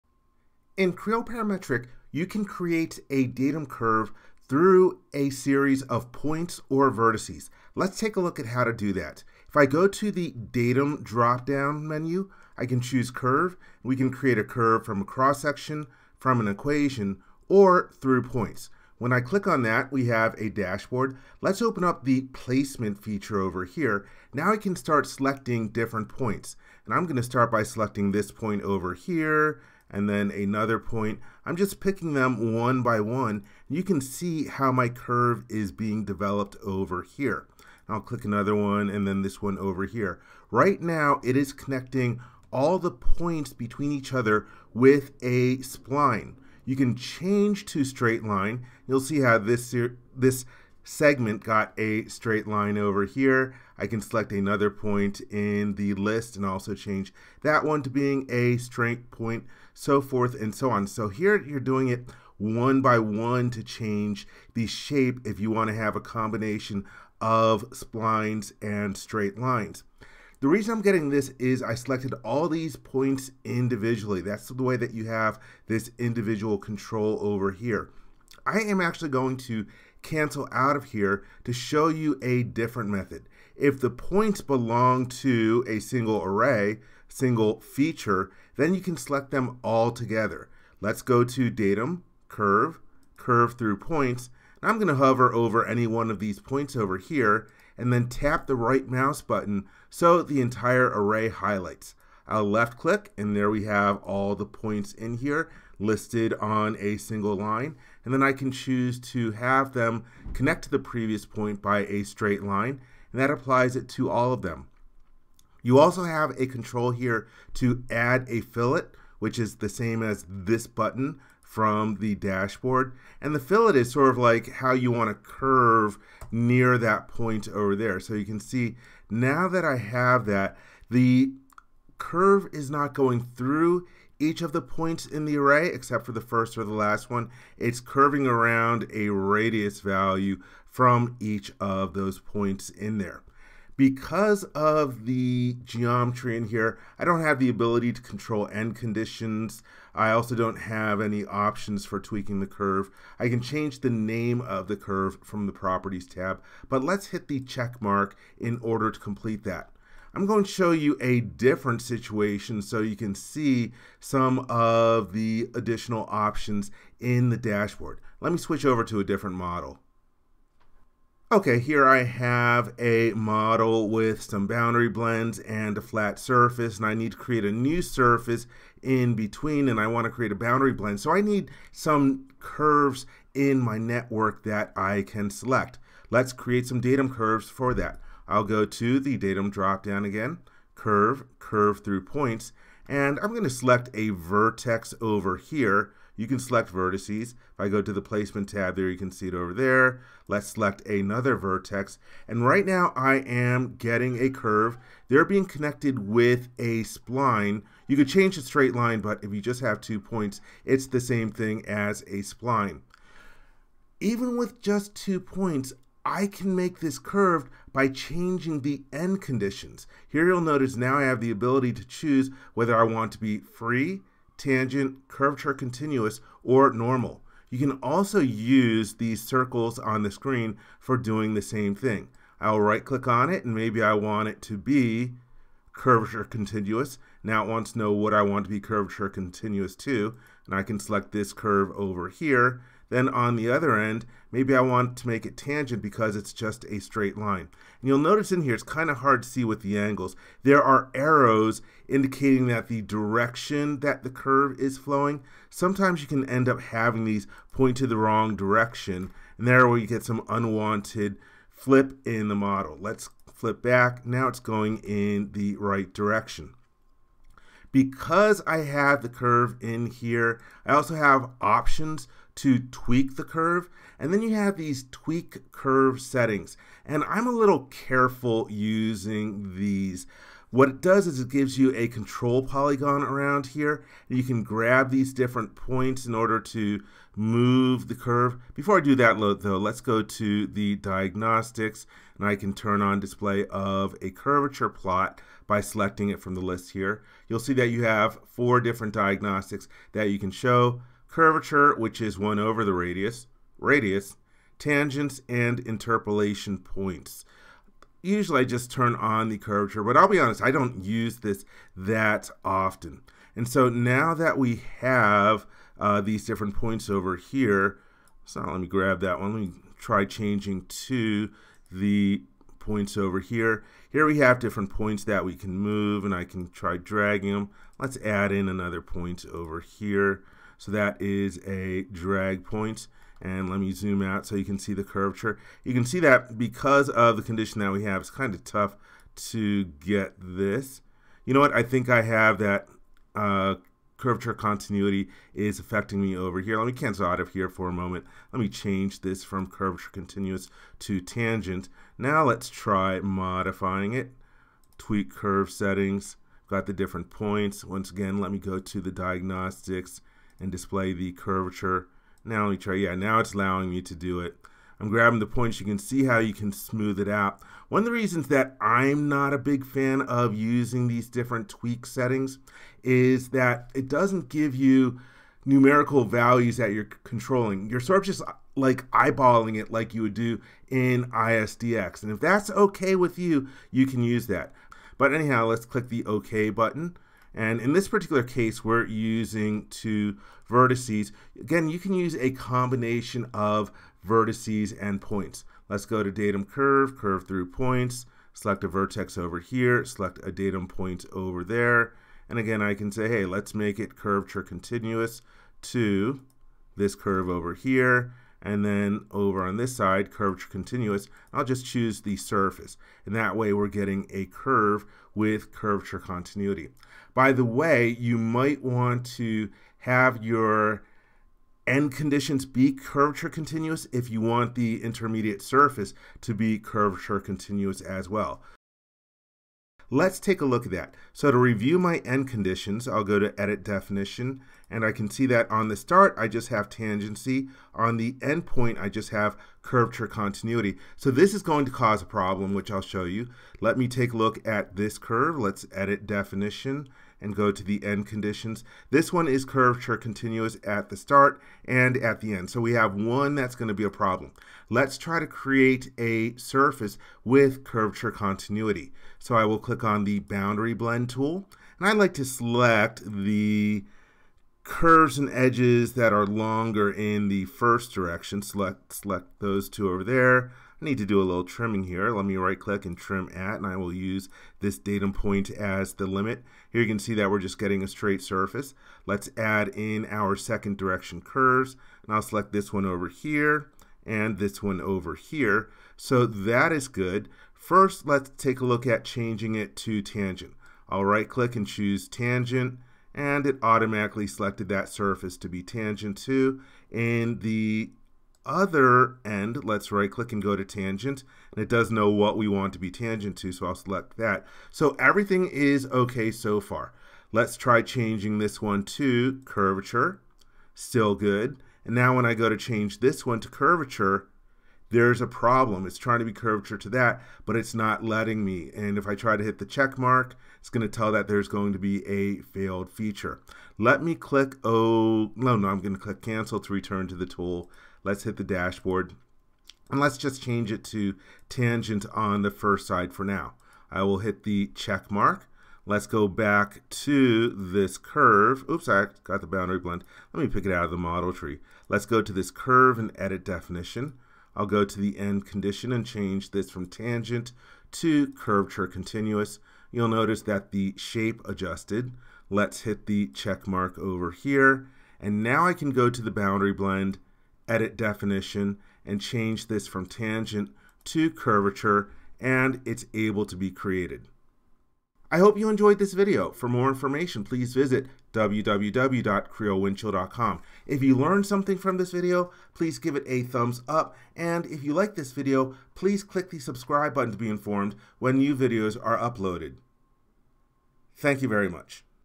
Creo Parametric - Curve Through Points | Datum Features
This Creo Parametric tutorial shows how to create a Datum Curve Through Points. It shows how to:
* use individual points, point arrays, and vertices.
* apply End Conditions and Tweak Curve settings.
* create a Boundary Blend.
* manipulate the curve end conditions to affect the surface end conditions.

Thanks,
Dave